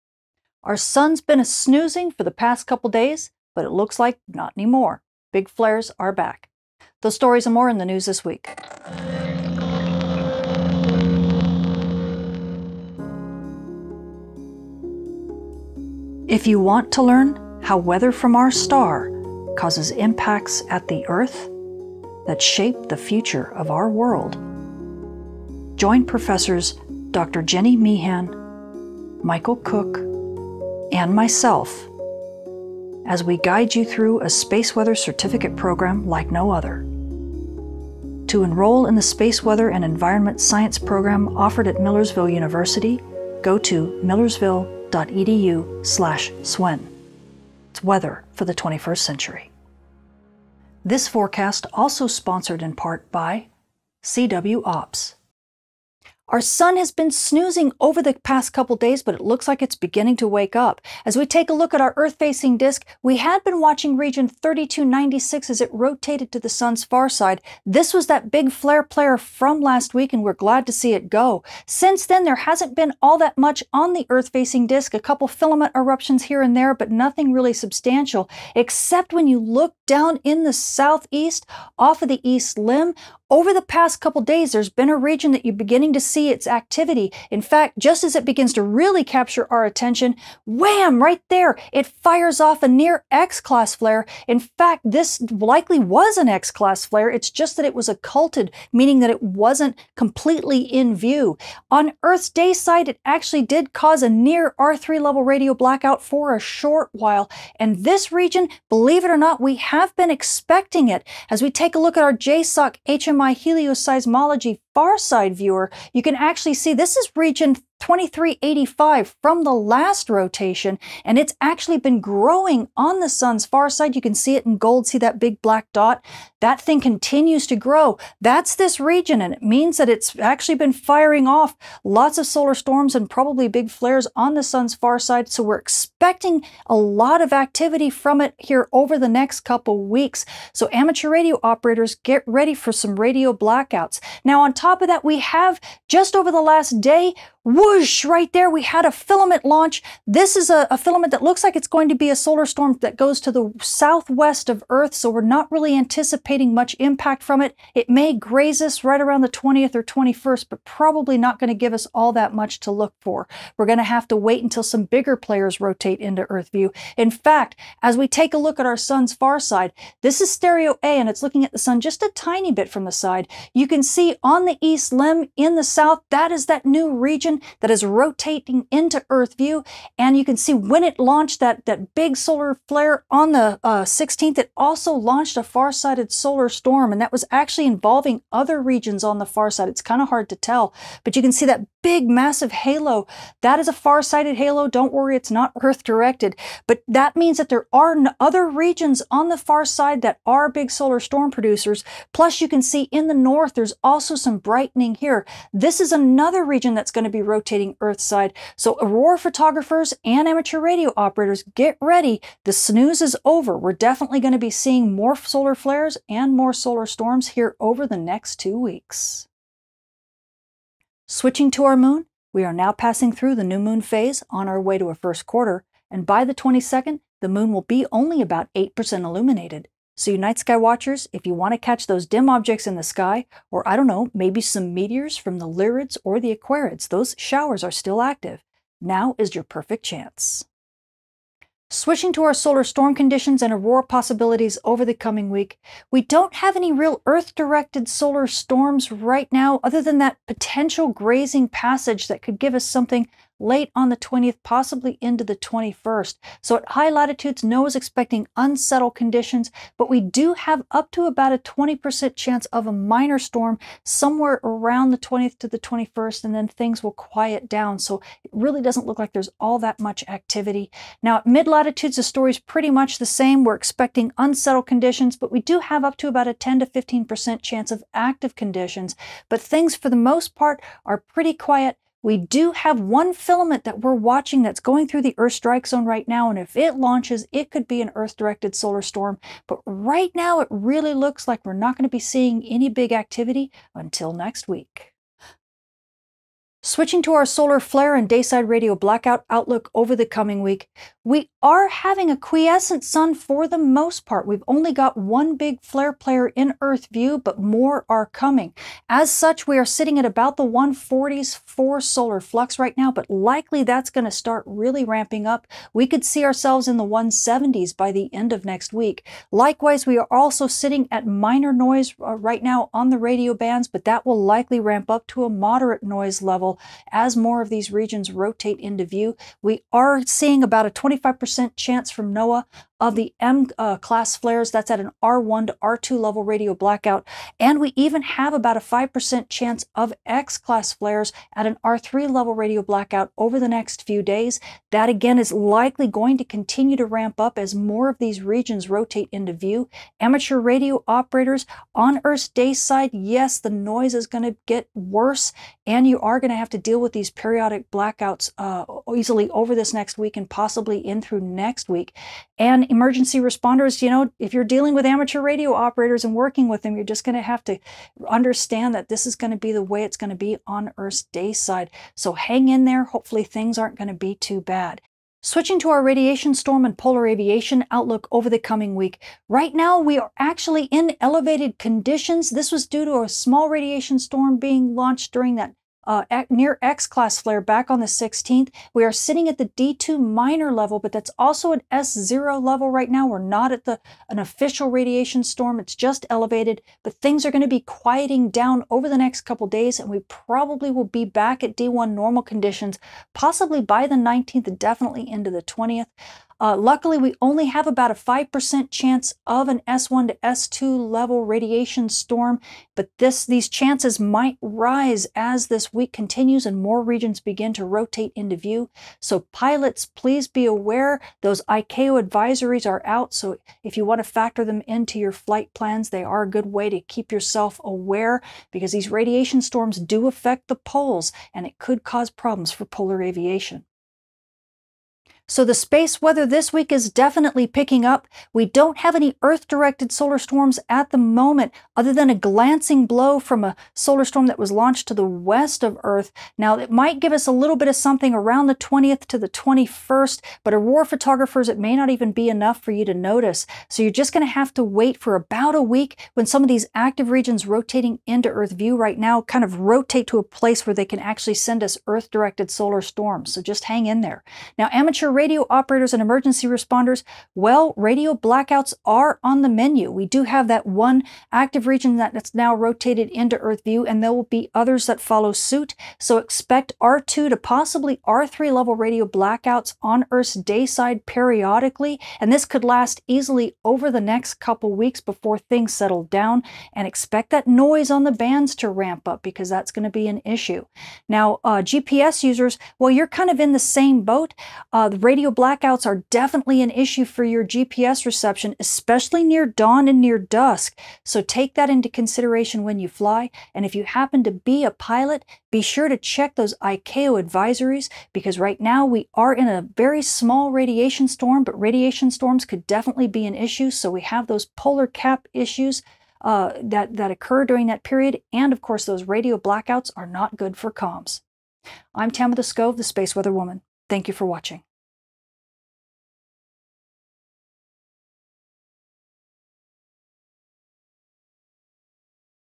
Goodbye Solar Snooze Big Flares are Back | Space Weather News 18 May 2023
This Space Weather News forecast sponsored in part by Millersville University:
After a few days spent napping our Sun wakes up with a new set of regions rotating into view. The first is region 3310, which has already fired a near X-class flare and promises to give us a good show over the next two weeks. Amateur radio operators and GPS users, expect radio blackouts to pick up again, which could interfere with signal reception on Earth's dayside. Aurora photographers will enjoy a bit more quiet time this week, with the possible exception of a slight disturbance on the 19-20th when a glancing blow coupled with a small pocket of fast solar wind could cause some aurora views at high latitudes. Impacts should be minor so likely we will need to wait until next week for more aurora activity. Learn the details of the new active regions rotating into view, watch the big solar storm launched on the Sun's farside, and see what else is in store this week.